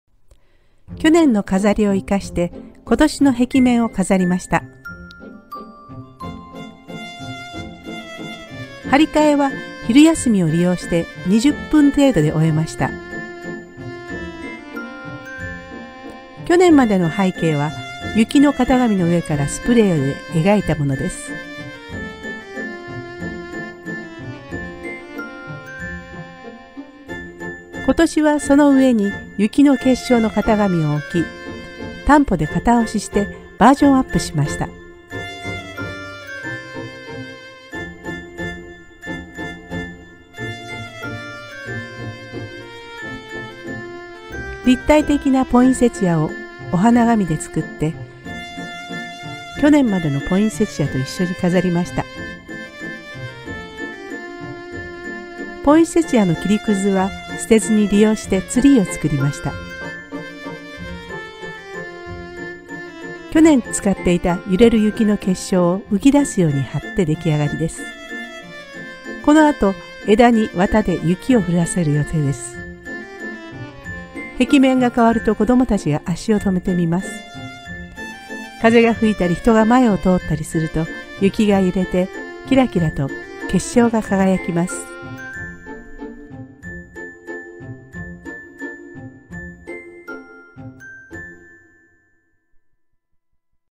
kimie gangi 12月の壁面飾り 去年から今年へ バージョンアップ
広い壁面を１から飾るのは大変です。昨年までの飾りで使えるものは残して、新しく付け加えたりレイアウトを変えたりすることで、今年の壁面を新しく、豪華に飾りましょう。
追加するパーツは事前に作っておき、貼り替えは一気に行います。
玄関を入ったところにある大きな壁面なので、子どもたちが足を止めて見てくれるのが、私を含め掲示委員会の子どもたちの喜びです。